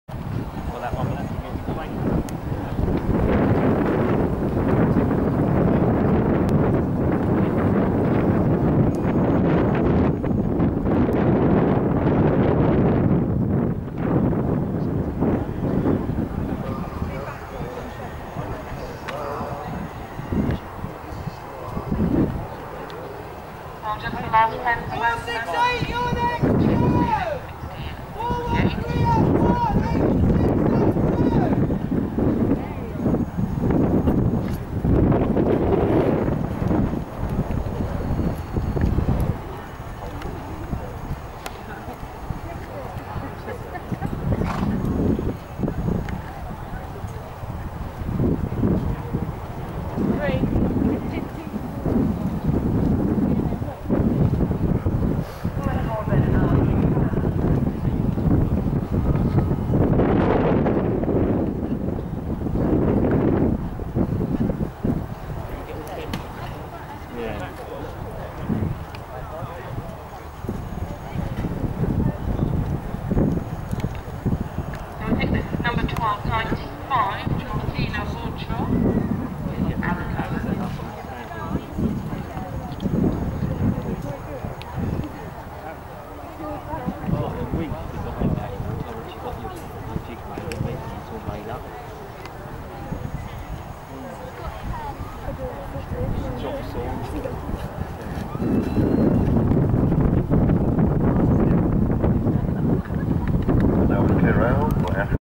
Angelina Z
Six year old mare. Double clear in Discovery GP at Arena UK